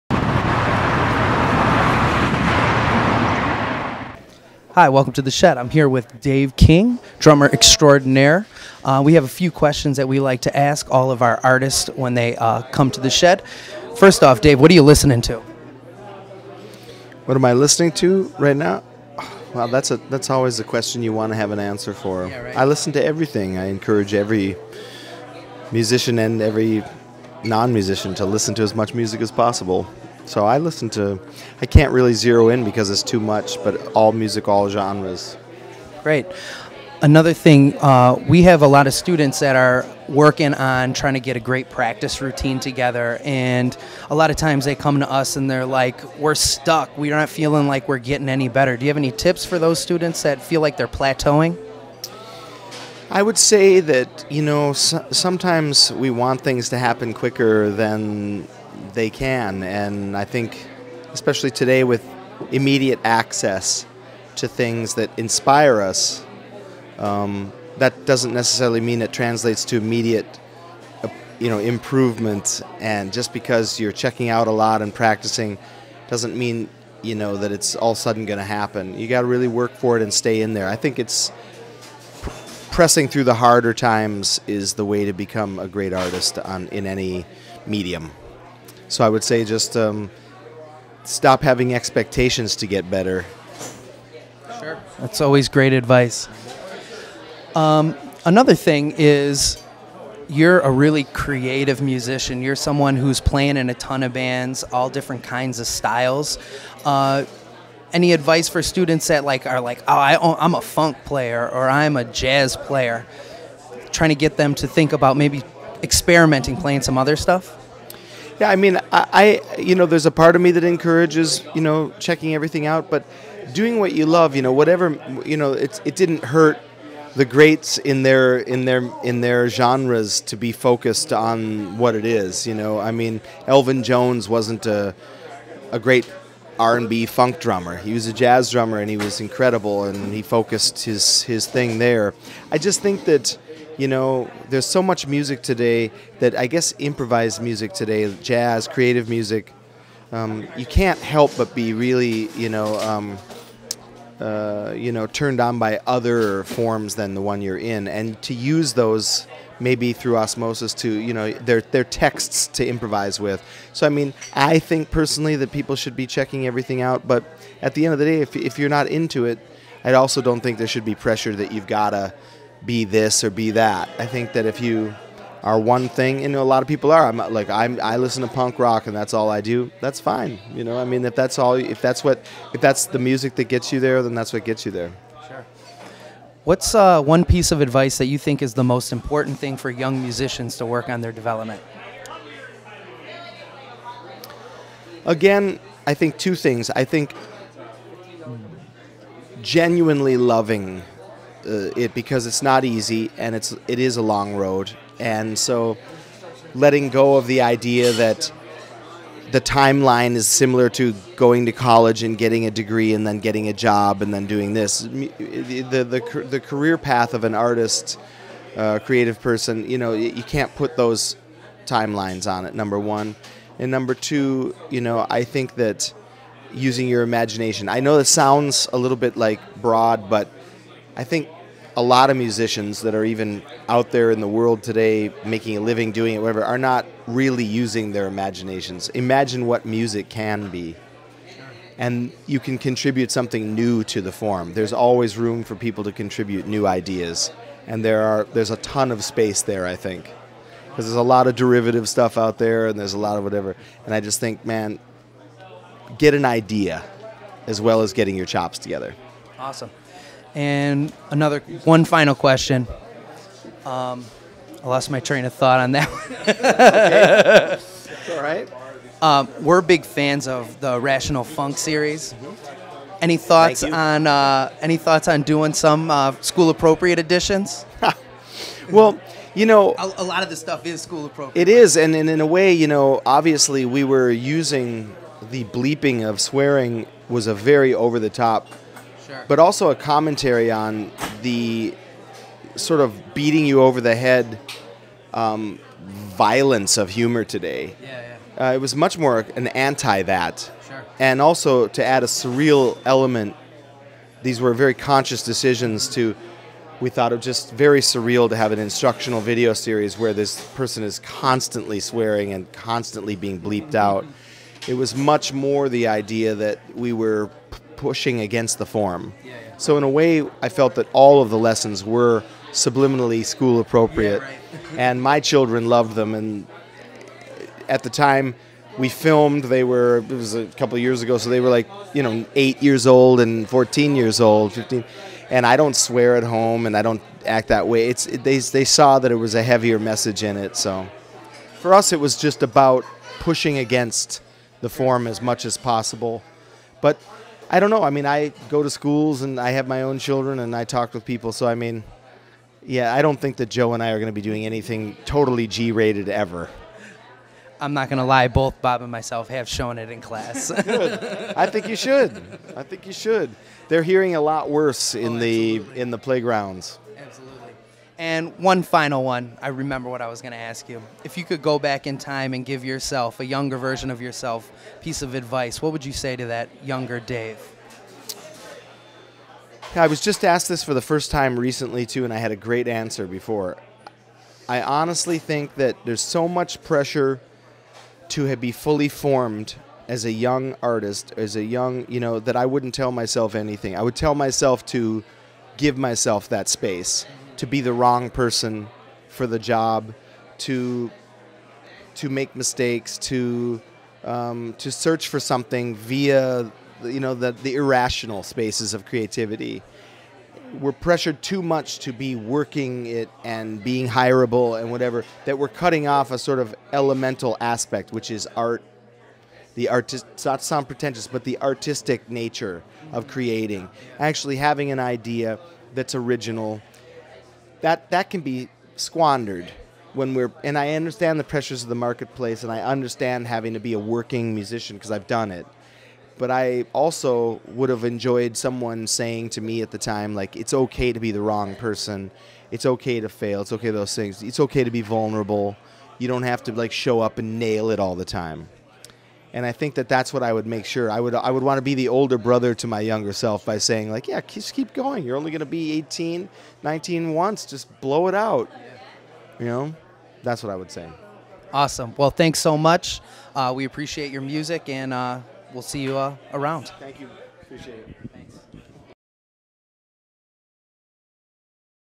Artist Spotlight #8: Dave King
We got a chance to talk to Dave King at The Green Mill.